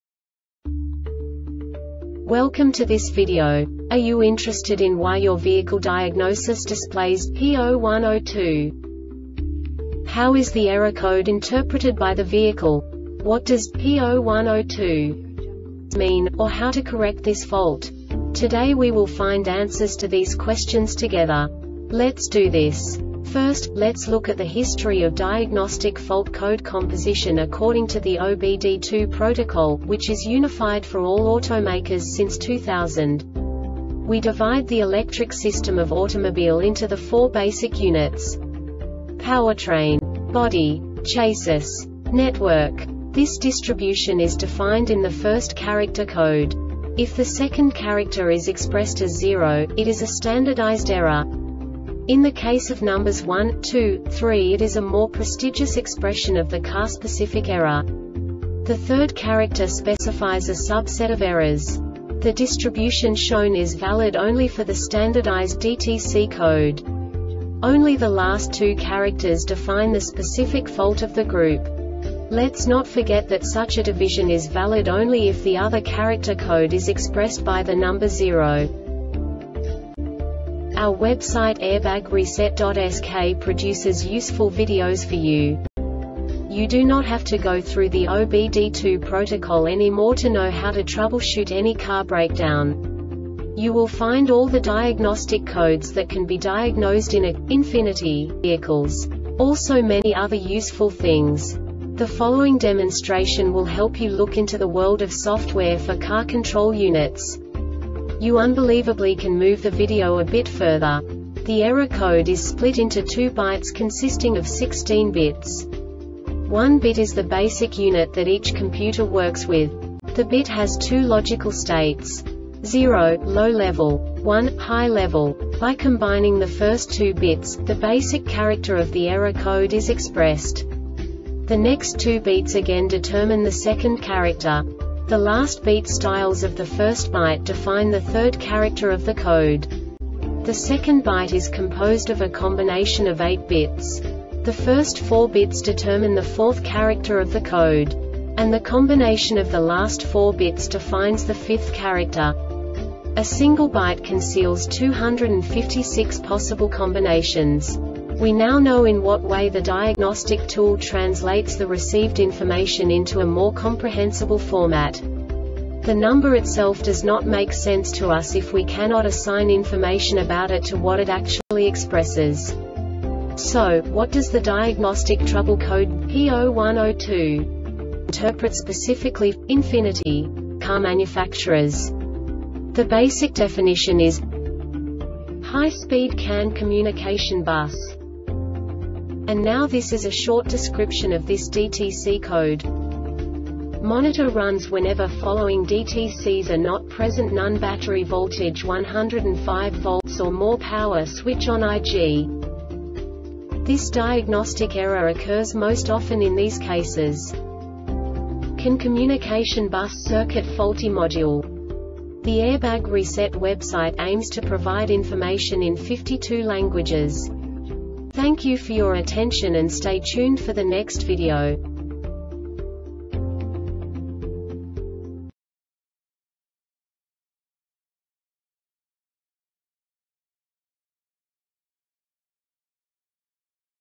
DTC Infiniti P0102 Short Explanation
Contents:
0:21 Basic DTC analysis according to OBD2 protocol standard.
1:48 Insight into programming
2:58 A diagnostically understandable description of the Diagnostic Trouble Code
3:05 Basic error definition

Make:
Infiniti
Code:
P0102
Definition:
High Speed CAN Communication Bus
Description: